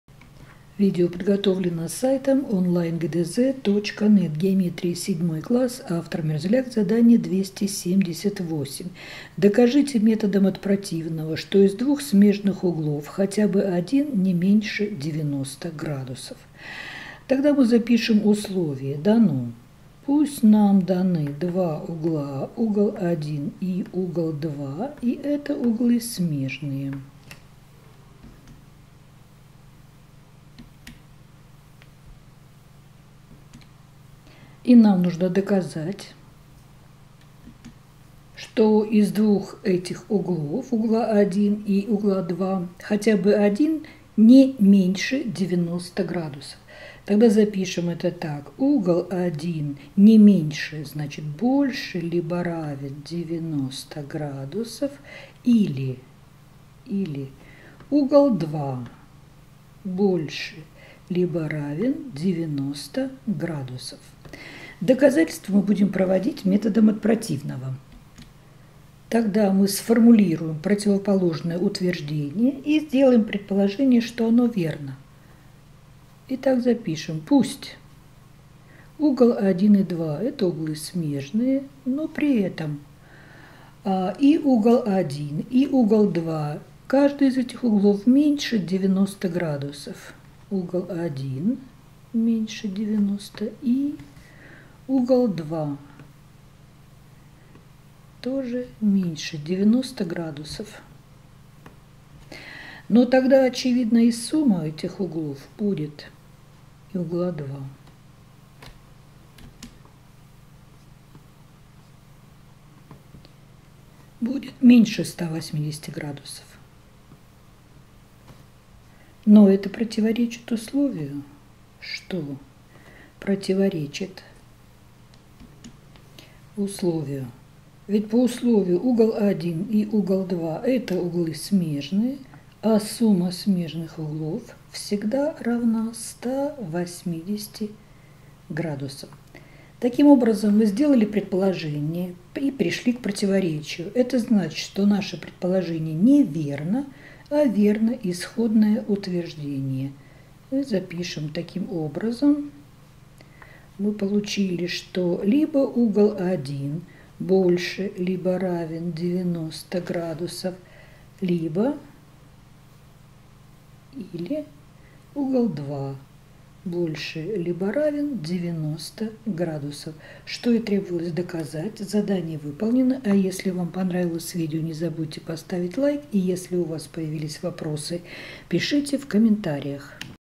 Задание №278 - ГДЗ по геометрии 7 класс (Мерзляк)
Для того, чтобы методом от противного доказать, что из двух смежных углов хотя бы один не меньше 90 градусов, необходимо предположить противоположное, - пусть каждый из двух смежных углов меньше 90 градусов. Тогда их сумма будет меньше 180 градусов, что противоречит основному свойству смежных углов. Таким образом, полученное противоречие доказывает истинность исходного утверждения.